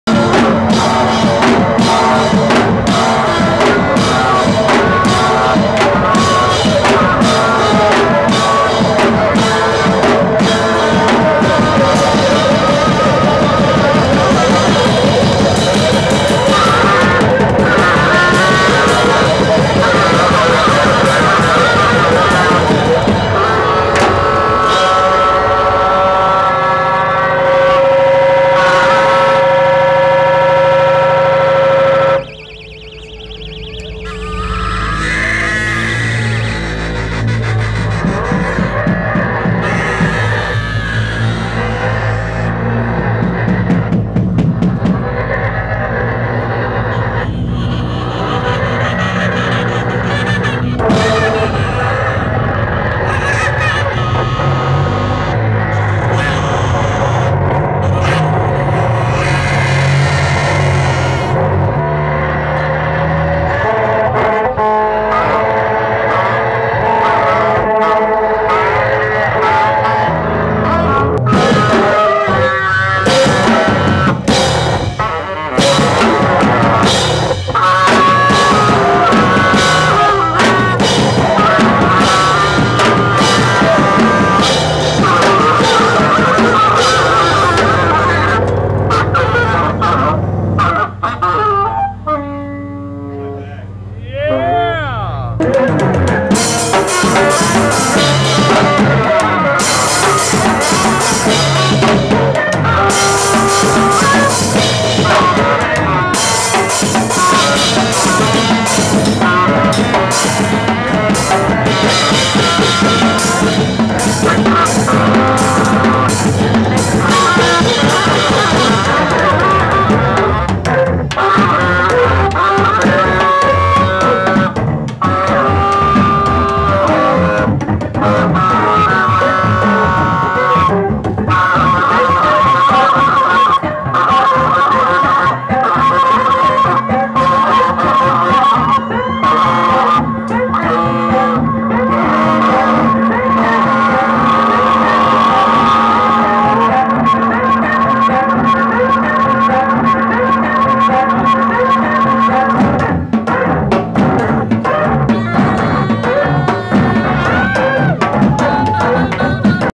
This Is My Condition & Dan Kozak at INC on February 16, 2008 at Churchill's Pub, Miami, FL
This Is My Condition & Dan Kozak (from Lawrence, KS) on Saturday, February 16, 2008 at the International Noise Conference at Churchills Pub in Miami, FL. --

Donation / Tip link if you would like to support my ongoing documentation activities or show appreciation for past work: